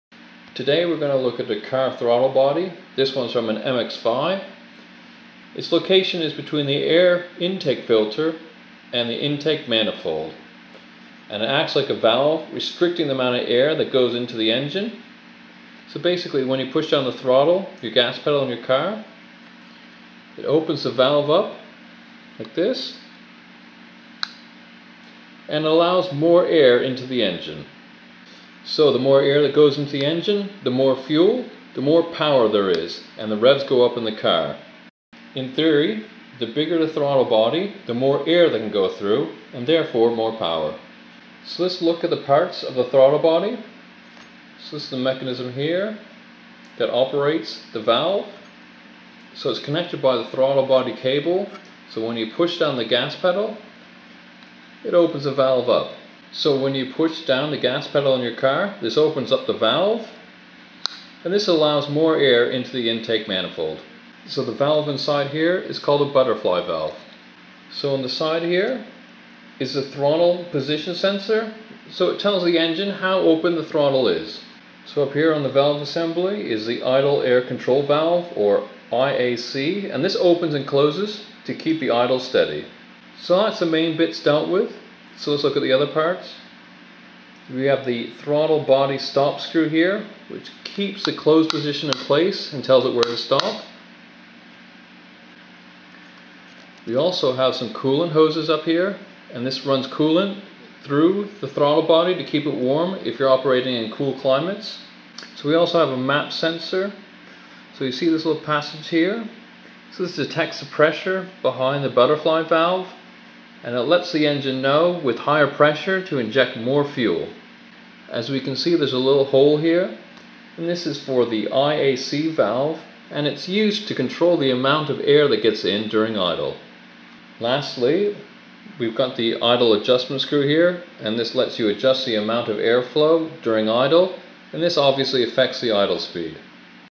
Throttle Body and Parts -Simple Explanation
What is a throttle body? How does a throttle body work? Simple and brief explanation. Off an Mazda MX5.